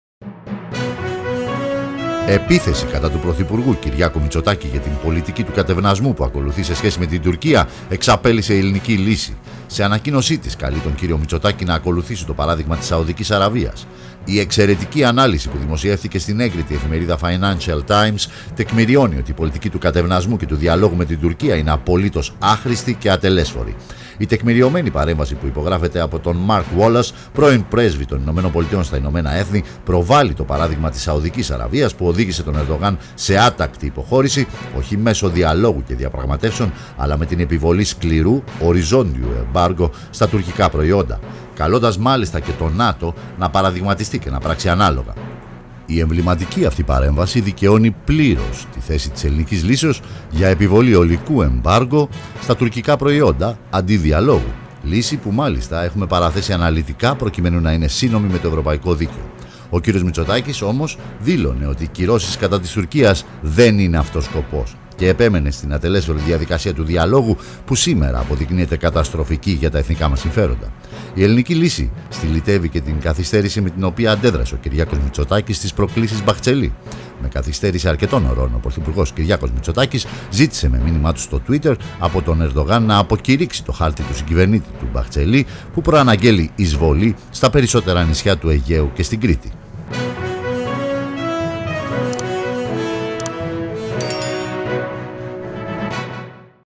Ελληνική Λύση: «Άχρηστη η πολιτική κατευνασμού. Ν'ακολουθήσουμε το παράδειγμα της Σαουδικής Αραβίας»
Επίθεση κατά του πρωθυπουργού Κυριάκου Μητσοτάκη για την πολιτική του κατευνασμού που ακολουθεί, σε σχέση με την Τουρκία, εξαπέλυσε η Ελληνική Λύση.